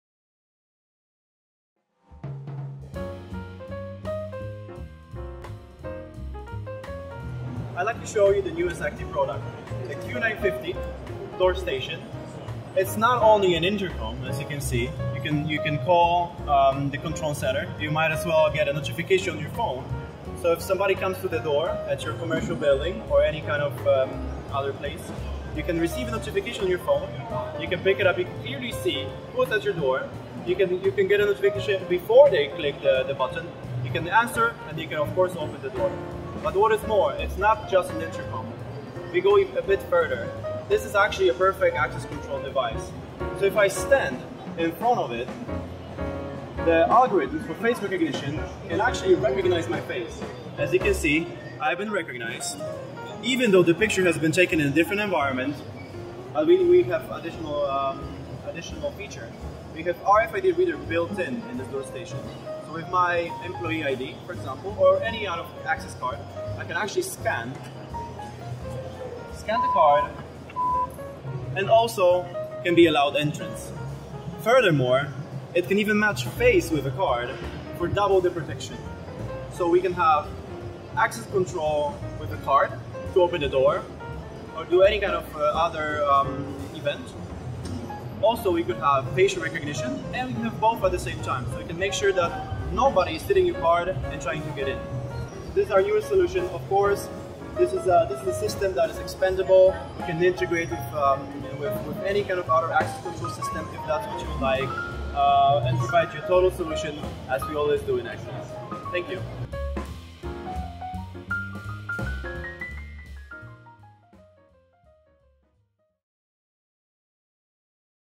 IFSEC 2017 Highlights _ Q950 7-in-1 Door Station _ EN
ACTi multi-functional door station provides an exceptional video and audio Intercom experience. A high-accuracy face identification technology is also used as an second layer of security along with access card detected by RFID sensor!

7 Key Features:
- Intercom
- Touch Button
- 6MP High Resolution
- Proximity Detection
- Door Control
- Face Identification
- RFID Sensor